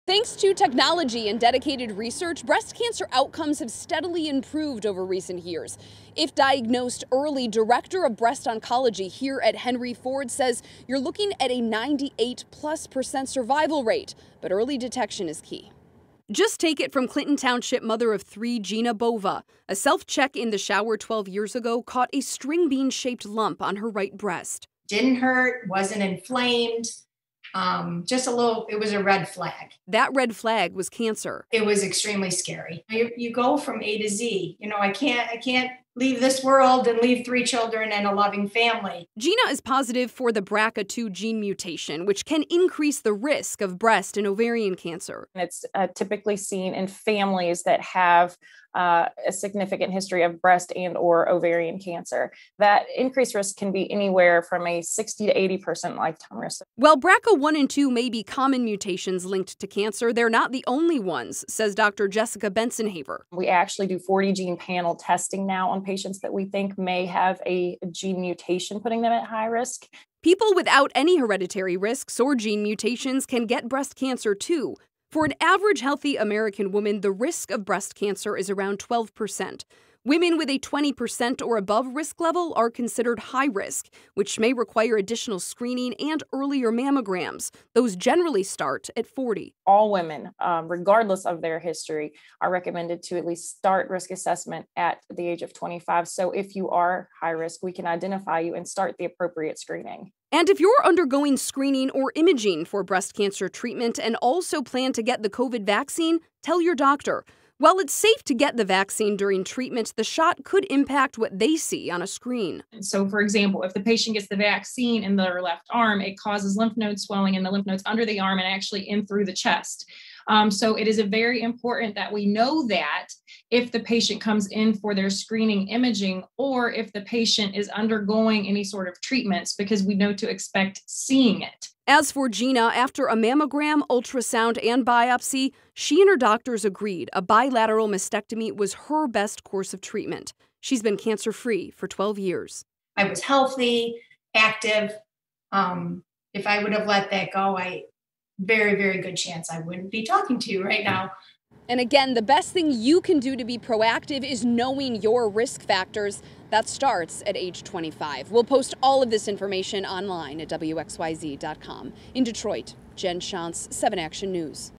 Women's Health Week: Everything you need to know about breast health
Thanks to technology and dedicated research, breast cancer outcomes have steadily improved over the past several years. If diagnosed early, Director of Breast Oncology at Henry Ford Health System Dr. Jessica Bensenhaver says patients can expect a 98%+ survival rate.